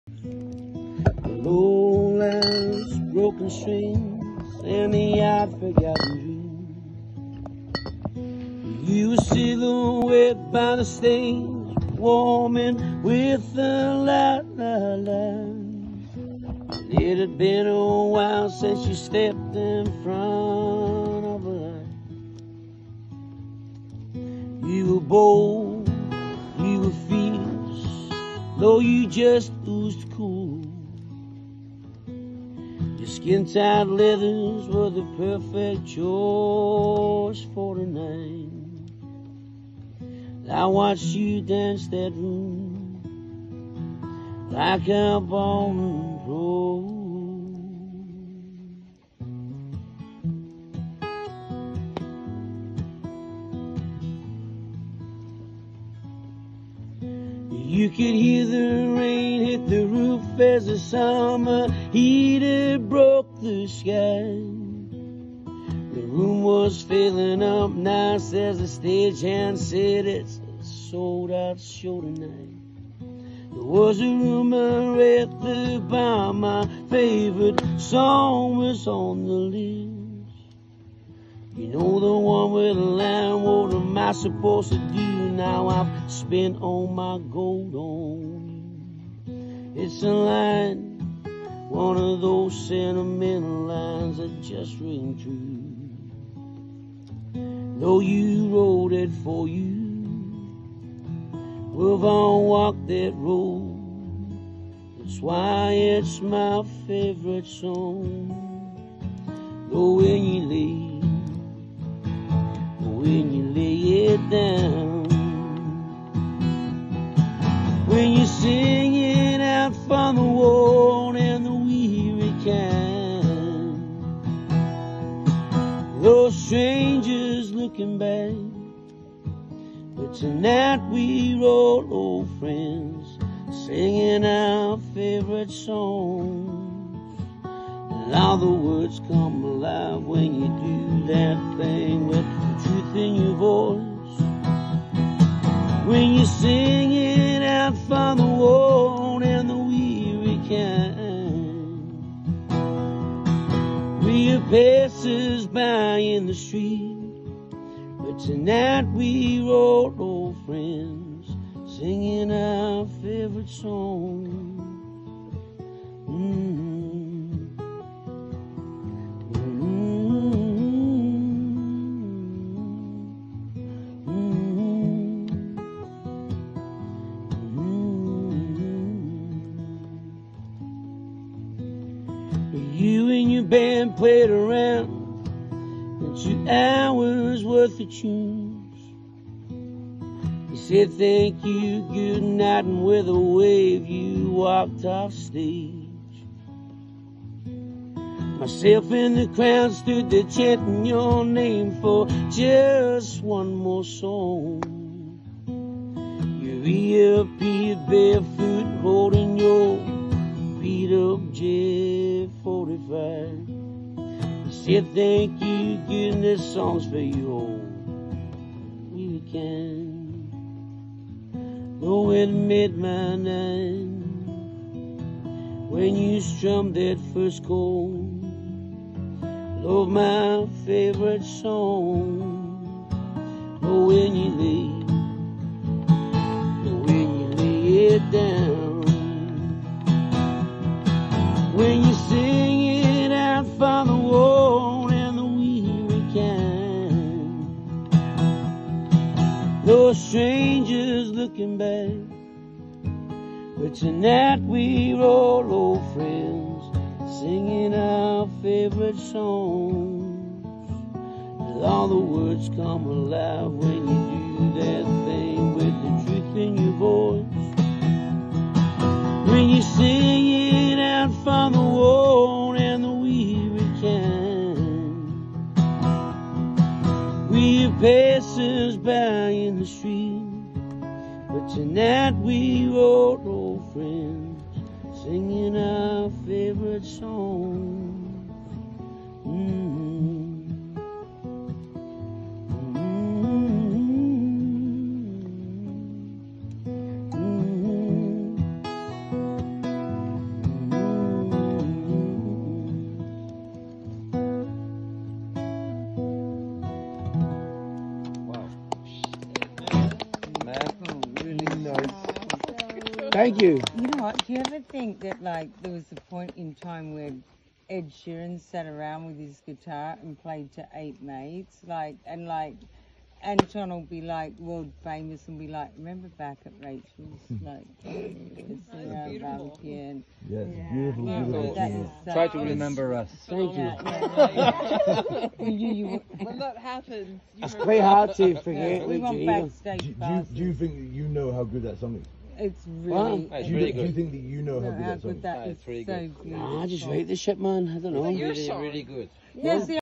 My Favourite Song
After party campfire vibes at my mate’s 40th. A grand wee night that was well worth the hangover.